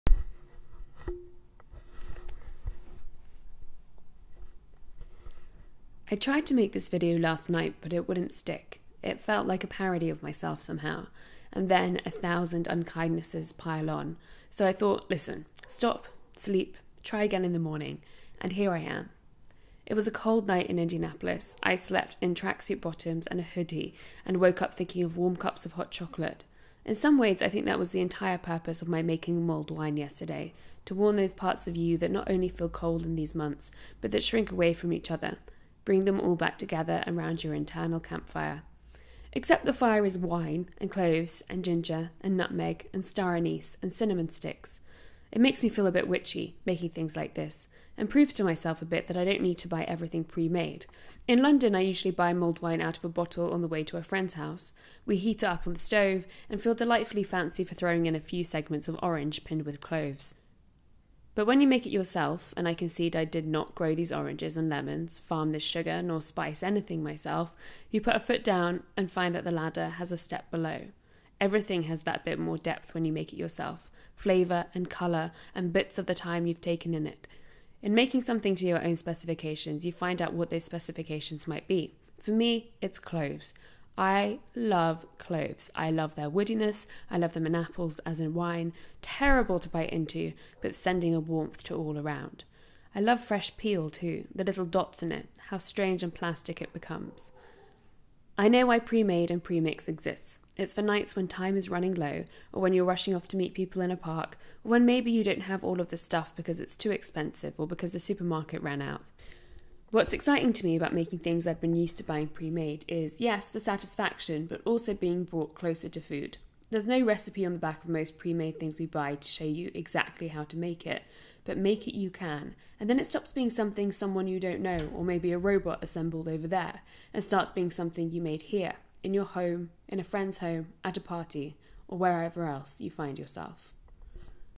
Mulled | Vlogmas Day 5
Making mulled wine, heating yourself up, and bringing yourself closer to food. Vlogmas Day 5 went up a day late but it still counts eh? :) Drink responsibly this festive season.